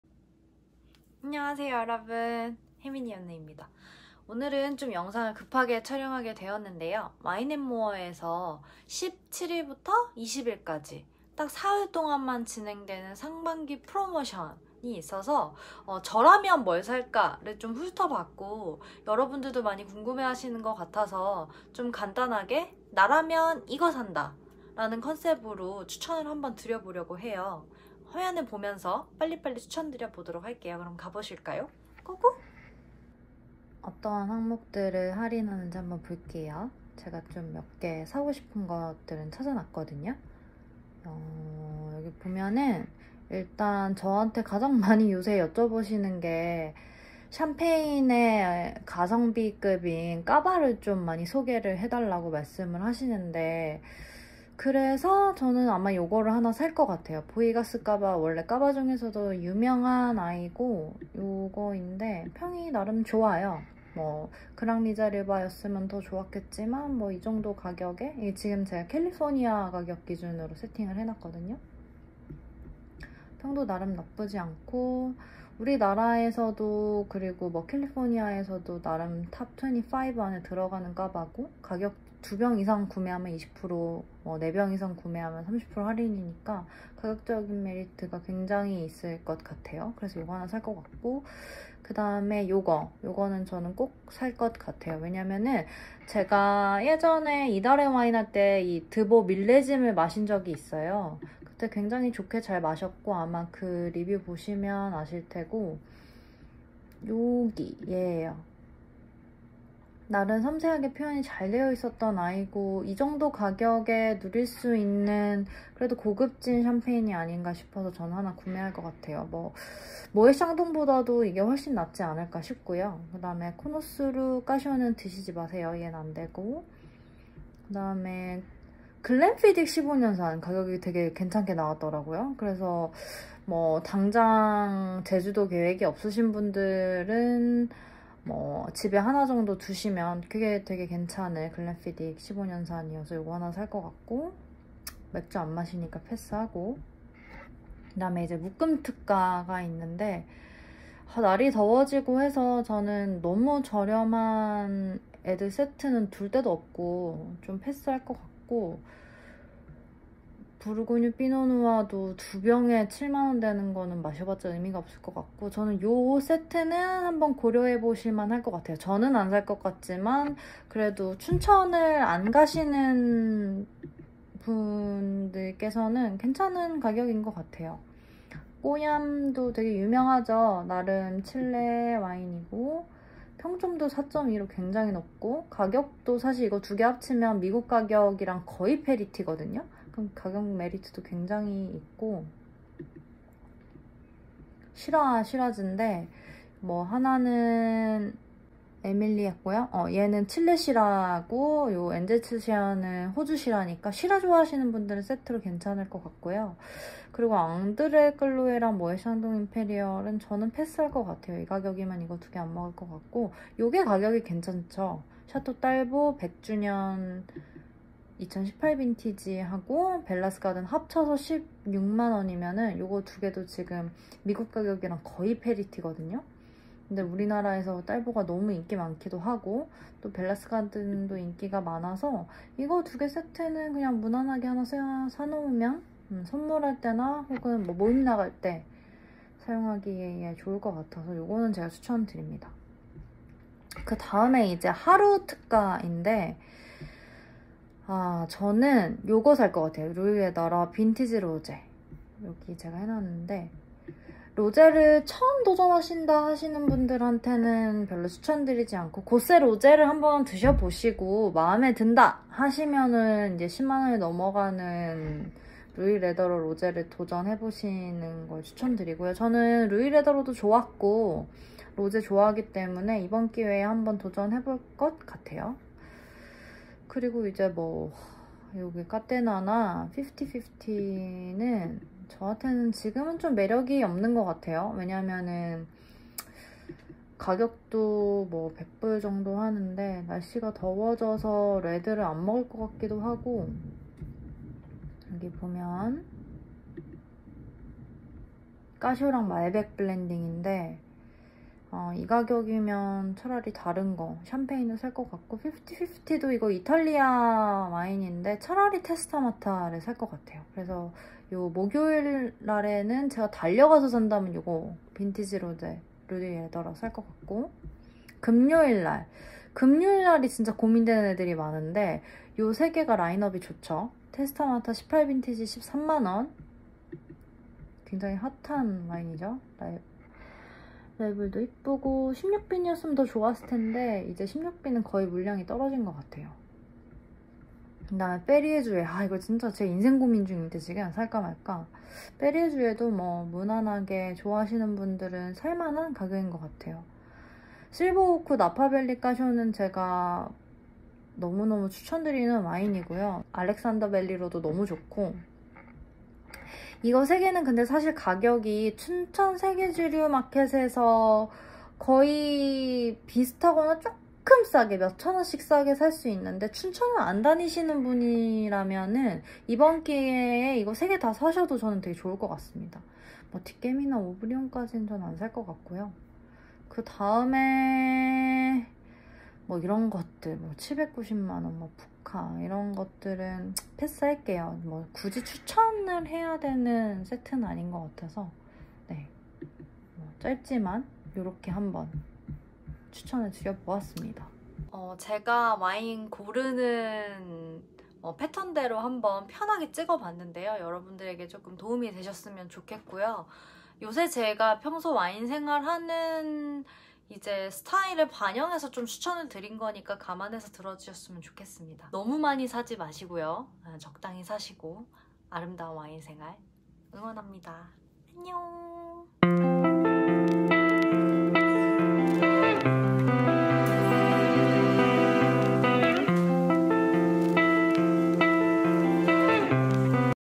[와인추천] 와인앤모어 상반기 바겐세일 | 나라면 이거산다!
안녕하세요 여러분 혜미니온느입니다! 😀 이번 영상에서는 상반기 와인앤모어 바겐세일을 맞아 나라면 ? 어떤 와인을 살 지 추천드리는 영상입니다.

와인에 대한 자유로운 의견 아래 오픈카톡방에서 나누고 있습니다. 와인 추천 받고 싶으시거나, 궁금한데 댓글은 쑥스러우시다면 여기 오셔서 저랑 이야기 나누실 수 있습니다! 오늘도 찾아주셔서 감사합니당 😍 혜미니온느 드림